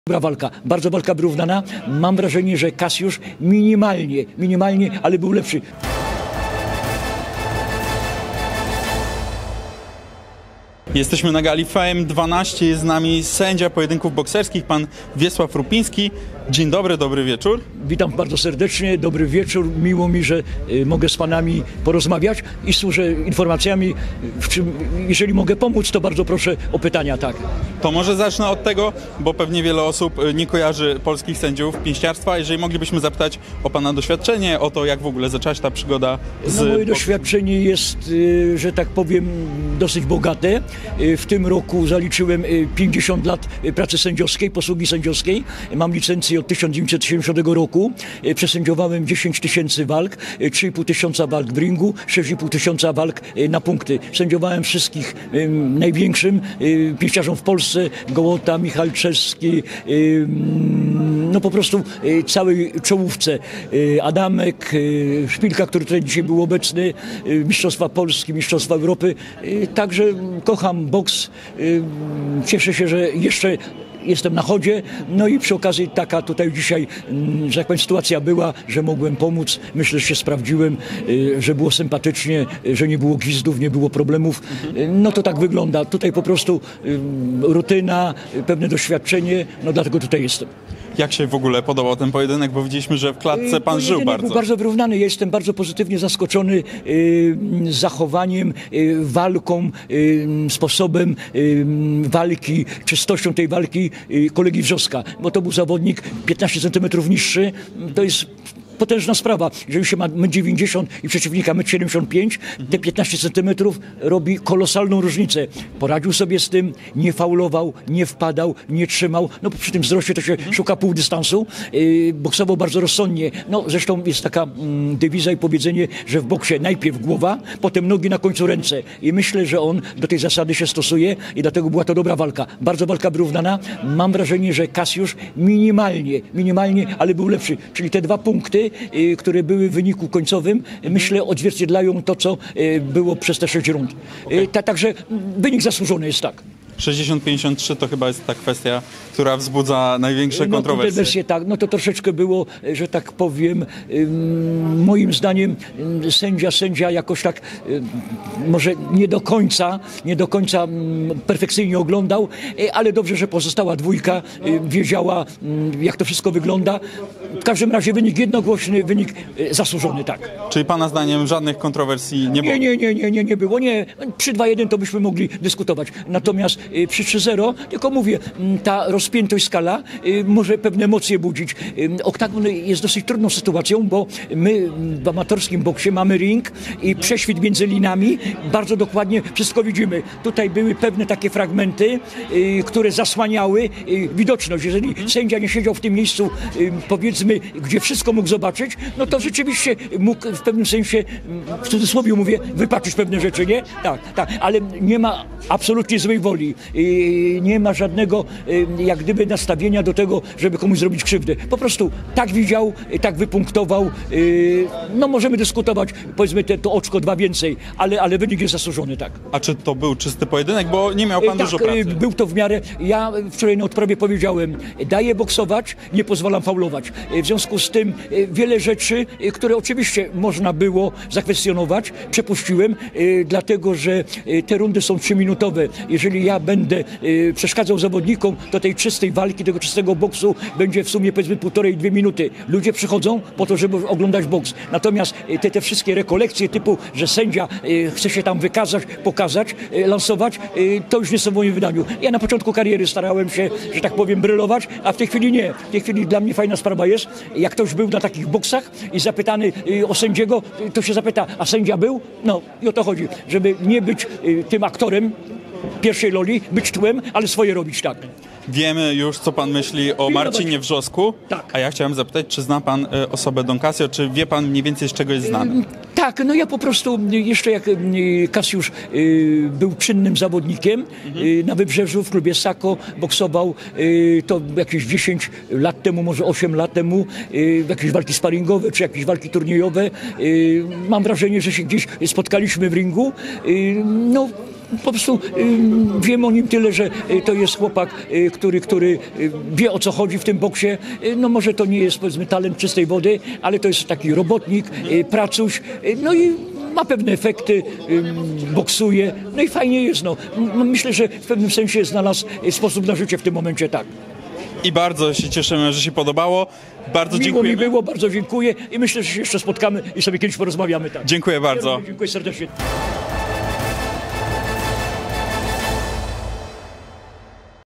SĘDZIA WALKI DON KASJO-WRZOSEK. UJAWNIA KTO WYGRAŁ
❌ To była z pewnością jedna z najciekawszych i najbardziej emocjonujących gal FAME MMA! Nie brakowało wspaniałych wymian ciosów, wielkich debiutów i bolesnych porażek. Nie zawiodły także main-eventy - Don Kasjo wygrał najpierw z Wrzoskiem, a później także z Boxdelem. O opinię na temat walki poprosiliśmy sędziego ringowego.